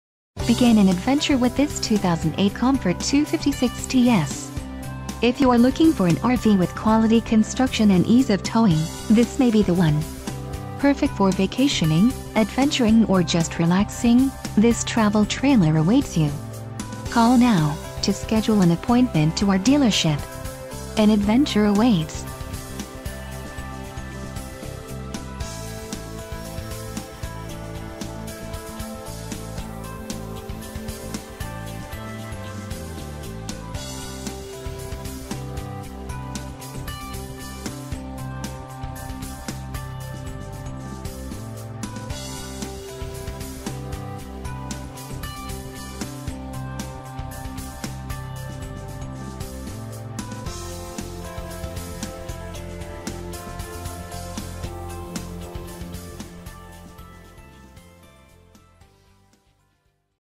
2008 Komfort 256TS
Save more of your hard earned money on this clean Used 2008 Komfort256TS travel trailer. Come make a deal with us at George M. Sutton RV where our customers are treated like family. Take a look at the equipment on this fabulous trailer and we hope to hear from you soon!2008 Komfort256TS Features:Exterior:C.S. AwningCable and Satellite Prep2-Entry DoorsStabilizer Jacks (4)Exterior GFIOutletExterior ShowerRear LadderWater HeaterSingle SlideoutEnclosed UnderbellyPower Hitch JackDual LP w/ CoverFront Window w/ RockguardDual Battery BoxesInterior:Dinette/ BedJ-SofaDucted Air ConditionerDucted FurnaceLiving Room Flat Screen TVSkylight in KitchenMicrowave Oven3-Burner Stove w/ Oven2-Door Refrigerator w/ Solid Wood Inserts2-Bowl Sink w/ Residential Style FaucetDay/ Night ShadesAM/FM/CD/DVD/MP3Power Bath FanMirrored Medicine CabinetShower Skylight